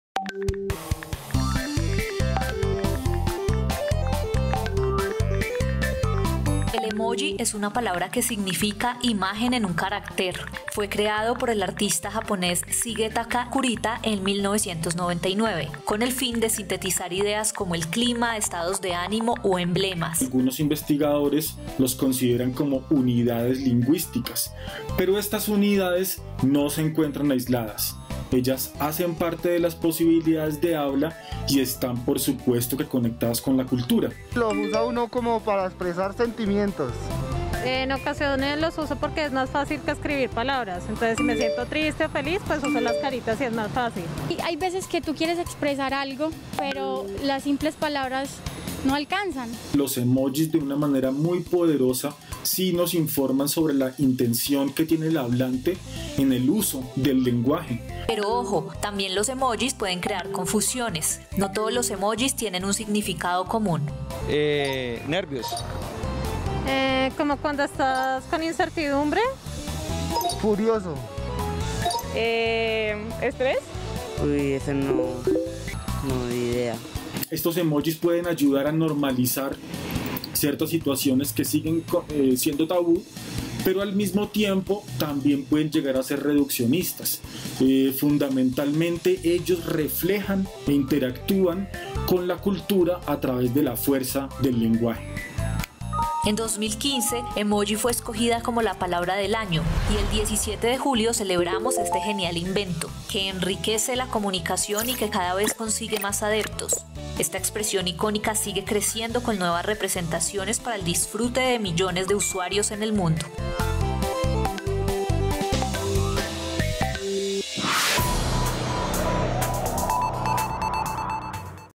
El Emoji. El simpático símbolo que cambió la forma de comunicarnos está de aniversario
El pasado 17 de julio, el mundo celebró el Día Mundial del Emoji.